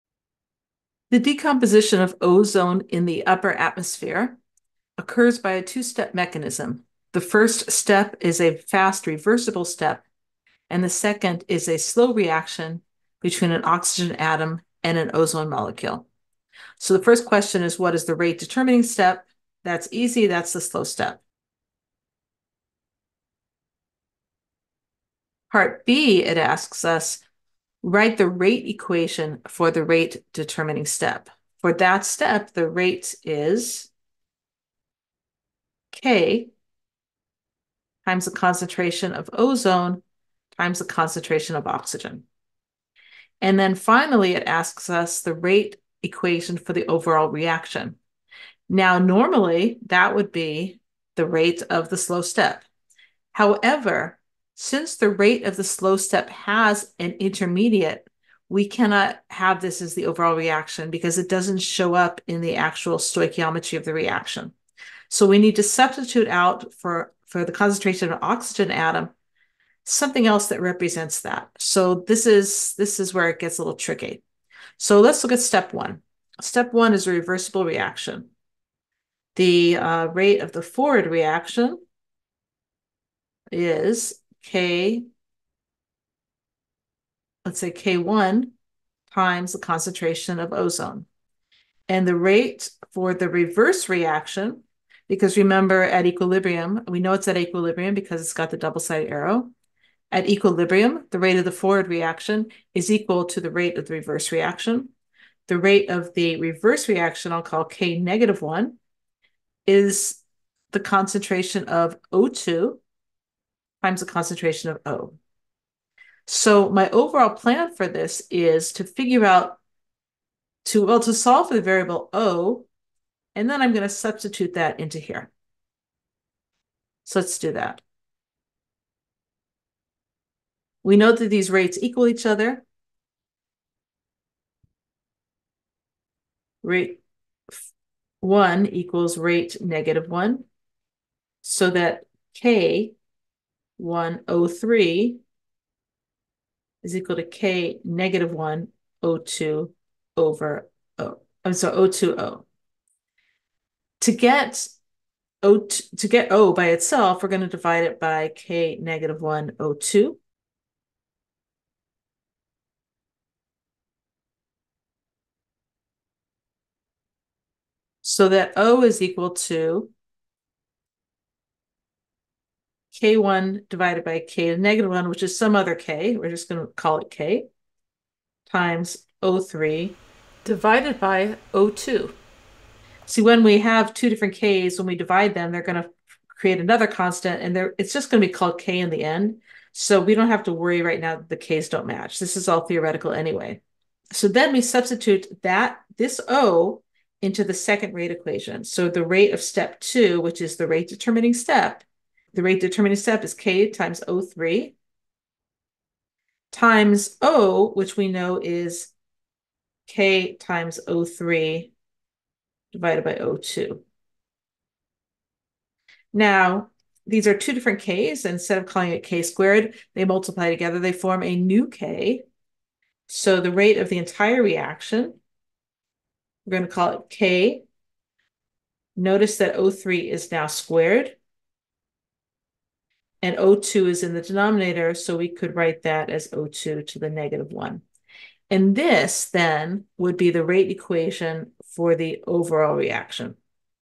Determining rate law from slow step of a reaction mechanism involving an intermediate
In this video, we use the slow step of a reaction mechanism to determine the overall rate law for the reaction.

Since the slow step involves an intermediate, we need to substitute the intermediate out to find the rate law in terms of only the reactants of the overall reaction.

Here is the problem we're solving:

The decomposition of ozone in the upper atmosphere to dioxygen occurs by a two-step mechanism.
The first step is a fast reversible step and the second is a slow reaction between an oxygen atom and
an ozone molecule:
Step 1: O3(g) = O2(g) + O(g) Fast Step
Step 2: O3(g) + O(g) --- 2 O2 Slow Step
a. Which is the rate-determining step?
b. Write the rate equation for the rate-determining step.
c.Write the rate equation for the overall reaction.